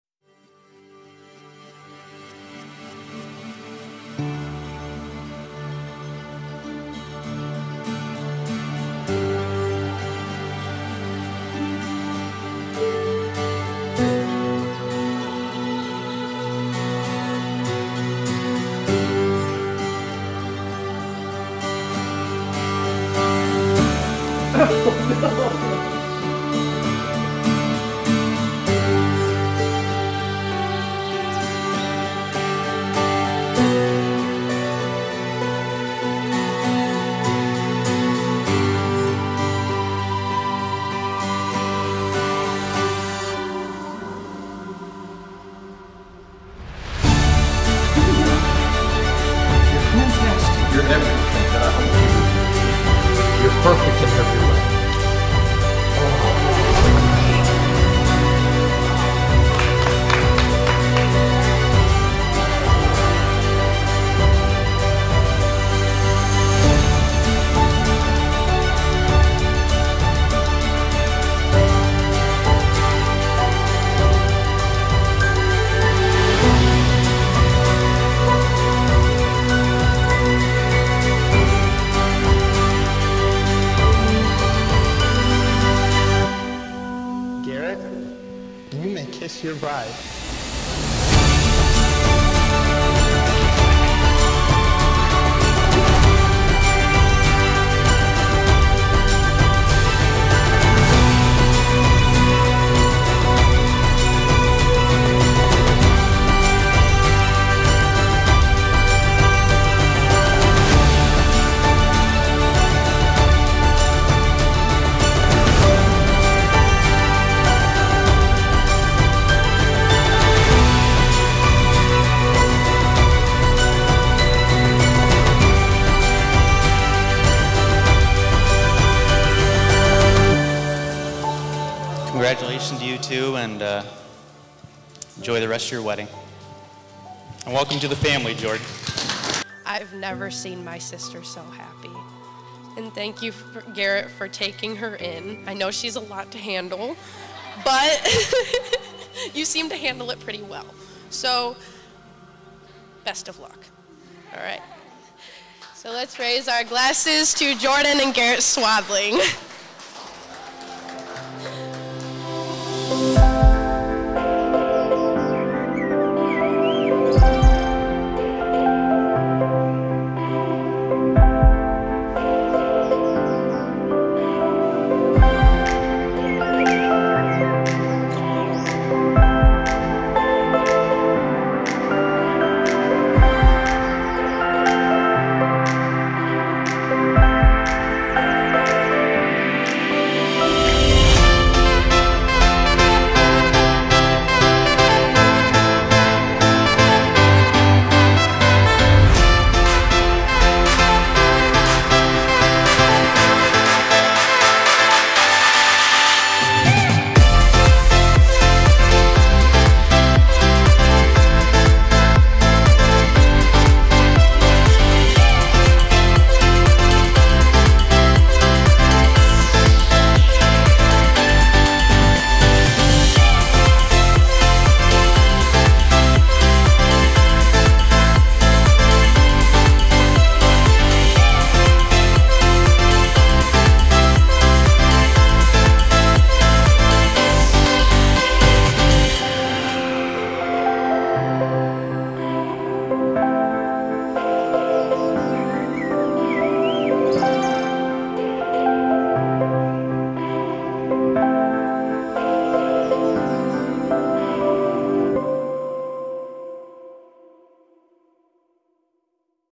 Jordan & Garret Wedding (Shot on Pocket Cinema Camera 4k with Helios 44M-4 lens)
This is a wedding I shot on the Black Magic Pocket Cinema Camera 4k in 2022. I also used a Helios 44M-4 for some of the shots. I've very pleased with the outcome!